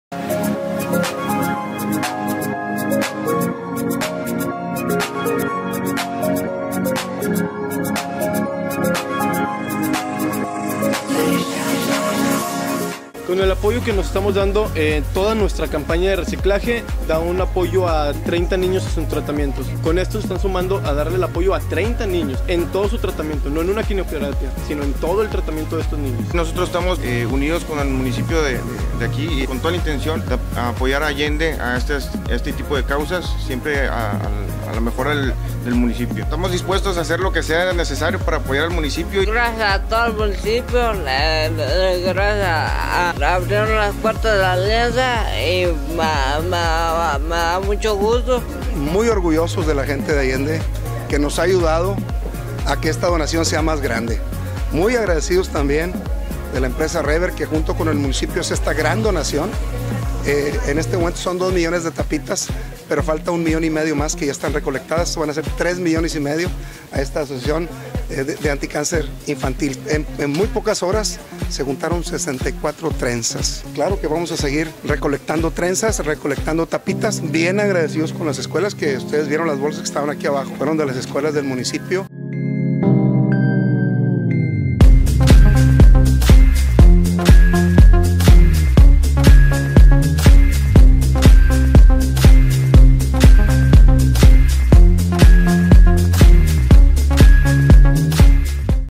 ALIANZA ANTICANCER INFANTIL.
Allende.- Por la tarde el alcalde Silverio Flores Leal, su distinguida esposa y su equipo de trabajo realizaron la entrega de taparroscas que se estuvieron recaudando a lo largo de meses atrás en diferentes puntos así como también escuelas del municipio, de igual manera las trenzas de cabello que mujeres y niñas estuvieron donando en el trascurso de la mañana, para la institución “Alianza Anticáncer Infantil”.
Por lo cual se mostraron muy agradecidos ya que con estas taparroscas beneficiaran aproximadamente a 30 niños con sus tratamientos completos, así mismo a las personas que donaron su cabello podrán realizarse pelucas para las niñas con este problema.